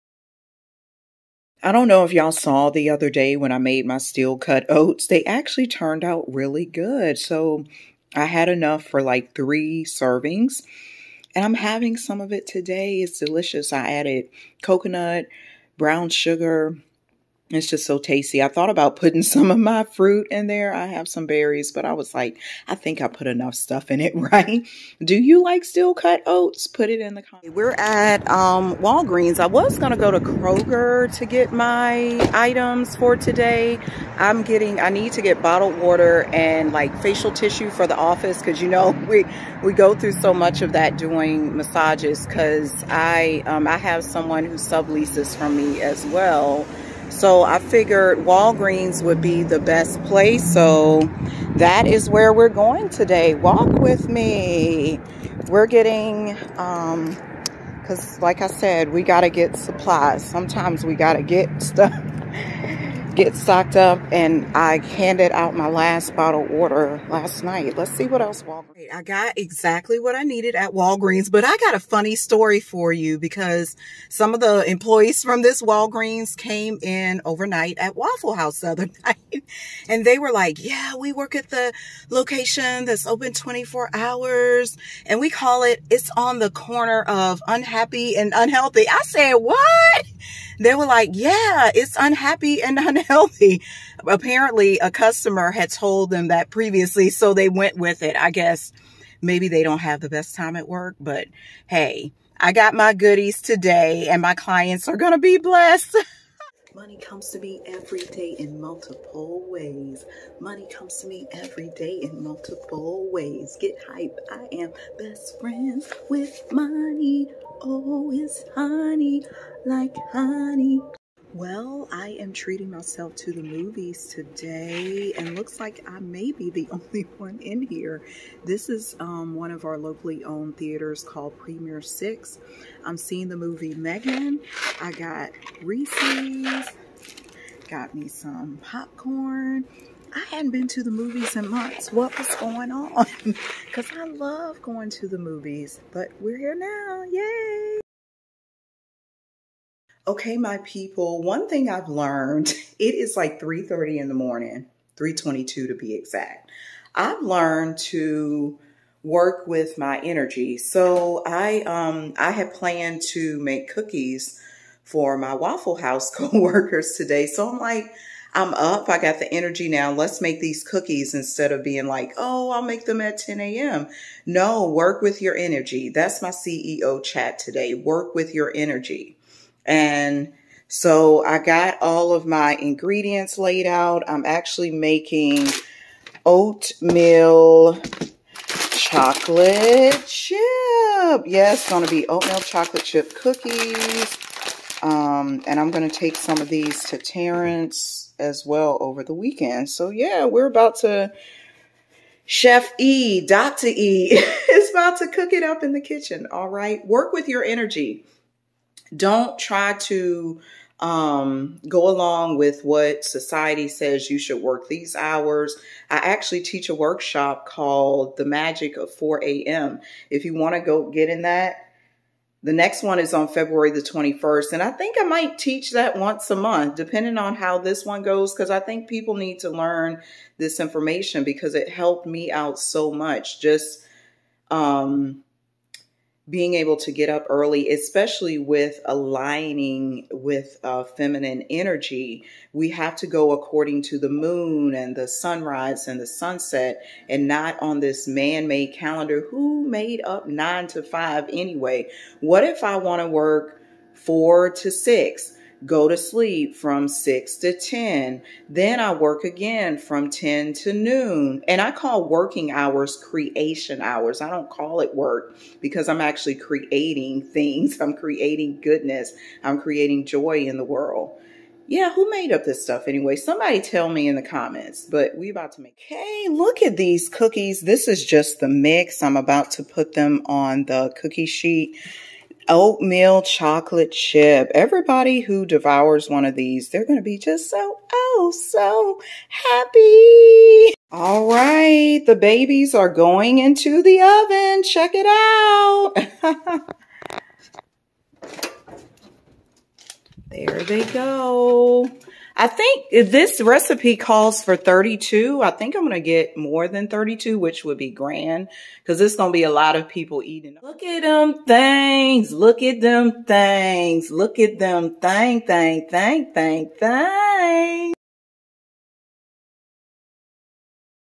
Ep. 30 Miss to Millionaire | Energy Flow, Baking Cookies
featured in this episode:

Walgreen's visit for massage office supplies
baking cookies for my co-workers

enroll in the Get Over Your Ex group coaching
heal your heart in 6 weeks
stop crying, start living
next session starts April 10th